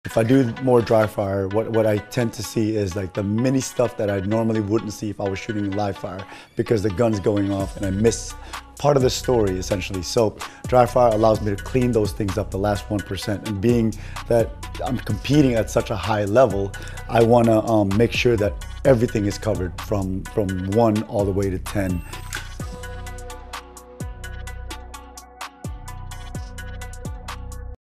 How dry firing makes you a better shooter!
jj Racaza explains how dry firing helps him work on his skills in between range times.